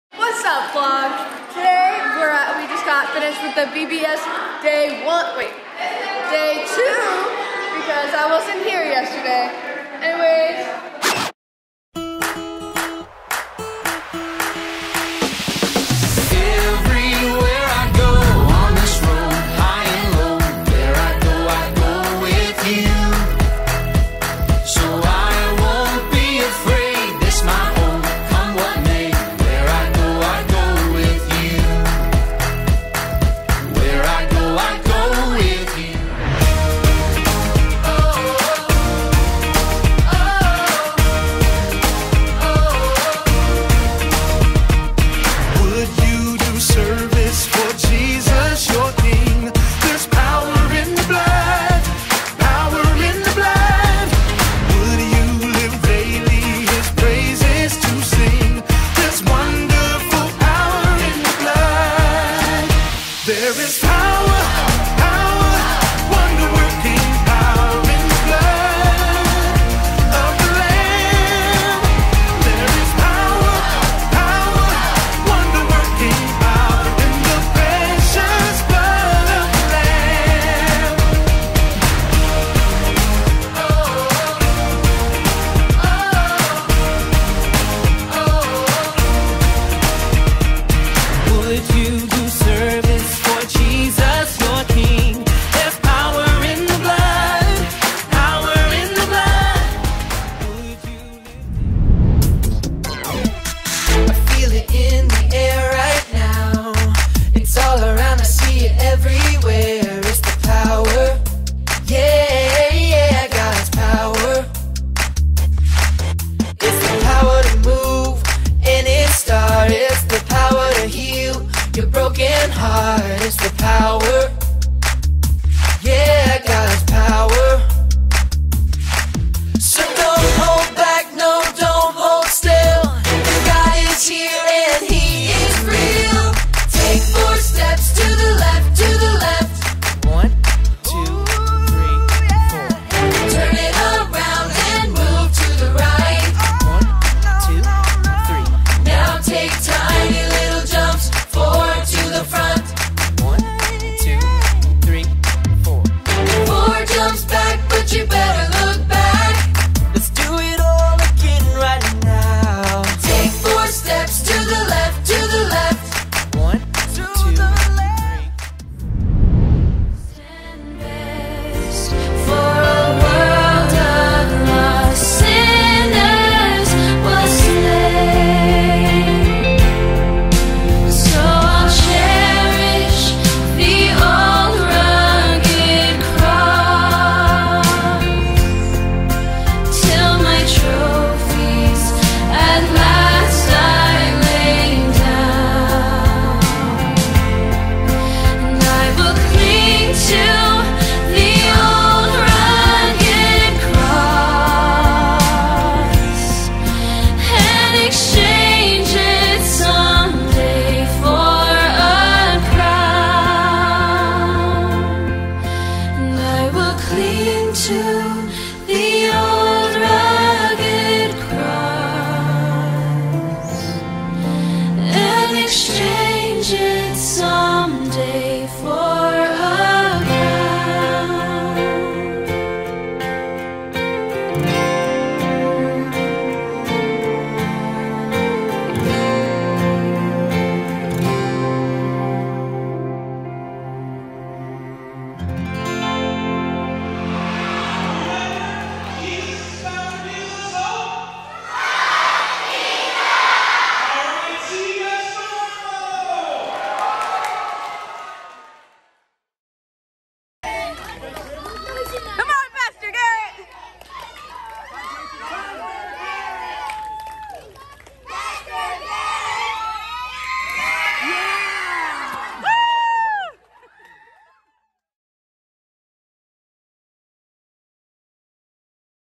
Vacation Bible School 2021 Recap
What a beautiful and fun week it was at Vacation Bible School this year! The theme of the week was Rocky Railway, Jesus' power pulls us through. After going through a year of isolation and separation, what a powerful message that theme was, because he did pull us through!

We hope you enjoy this snapshot of what VBS was like this year! Thank you to everyone who participated and served during the week. You all showed and shared the love of Christ this week!

Trust Jesus! His power pulls us through!